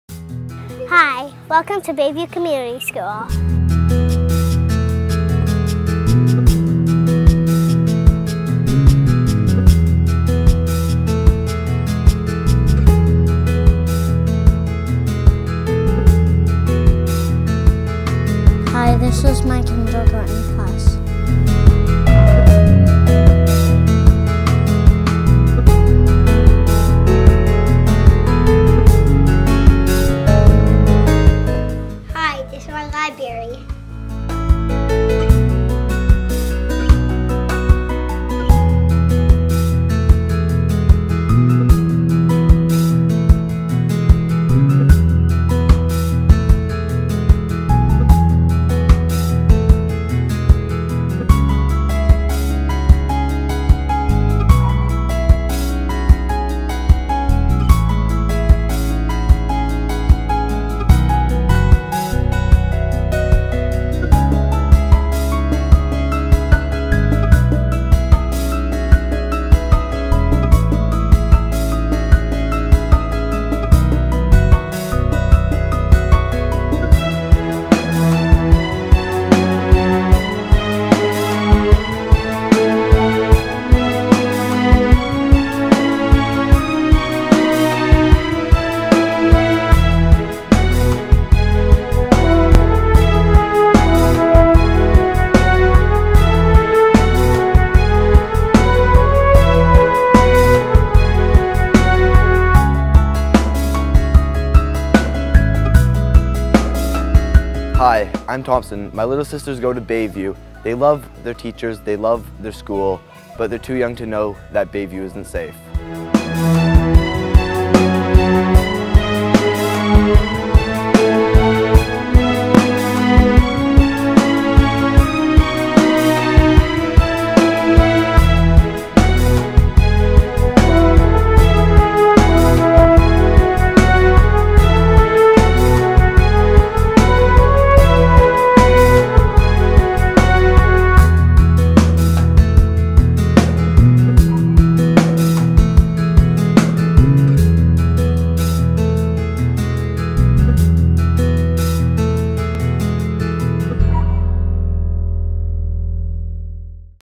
Bayview Community School Seismic Upgrade Action
Sign the Petition In November 2004, British Columbia's Premier Gordon Campbell made a $1.3 billion commitment to seeing all schools brought up to acceptable seismic life safety standards. However, work on our schools continues to languish while other provincial seismic mitigation efforts have been completed. The City of Vancouver has seismically upgraded water supply systems and bridges while the provincial electric utility, B.C. Hydro, is systematically upgrading its buildings and infrastructure. The provincial prisons were seismically upgraded and even provincially run liquor stores have been upgraded. Schools have not been seen as a high priority.

Many of our children's schools, like Bayview, are built of brick from the early 1900's. Because of this non-reinforced masonry, these schools would be the MOST dangerous buildings to be in during an earthquake. Bayview Community School Seismic Upgrade Action.  Please visit the Bayview Pac Website to learn how you can help